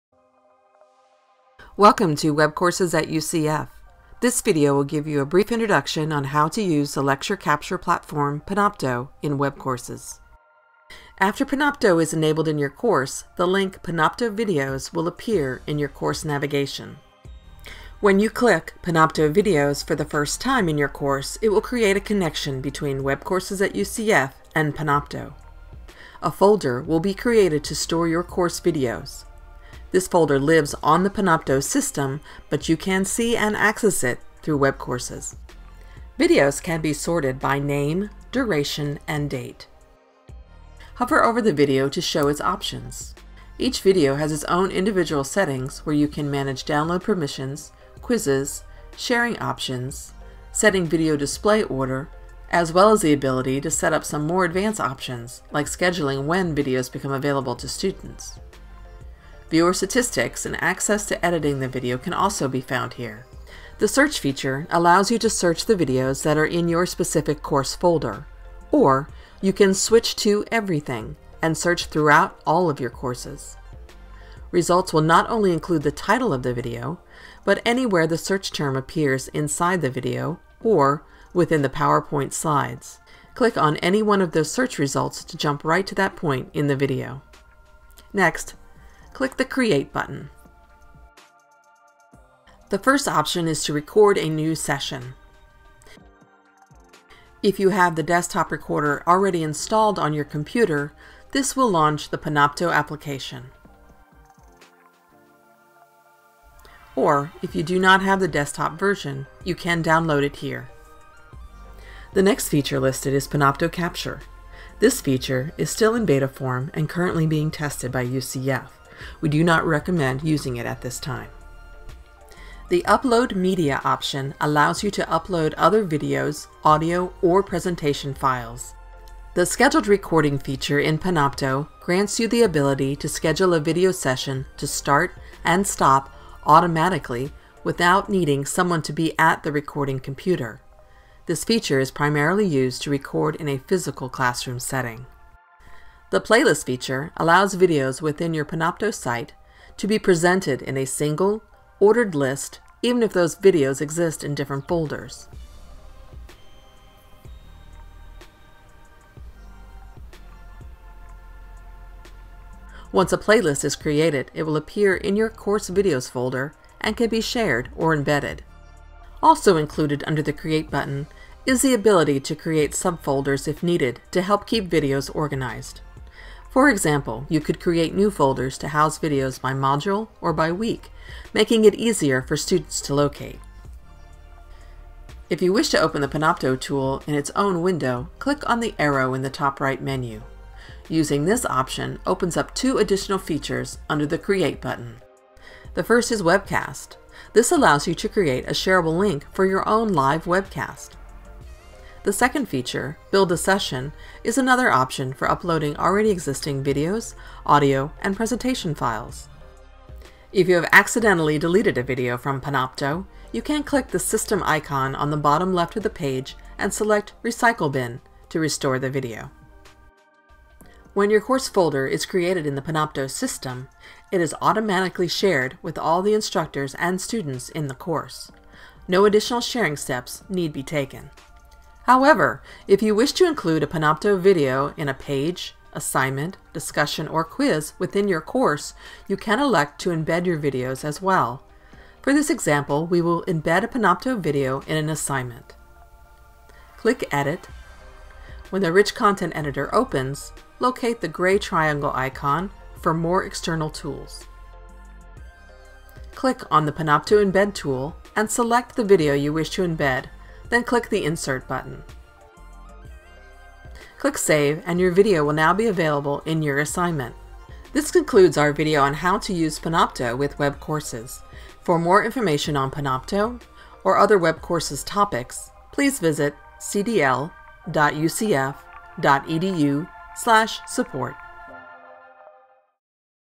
How to Use Panopto in Webcourses
Learn how to use the powerful lecture capture platform, Panopto, within Webcourses@UCF.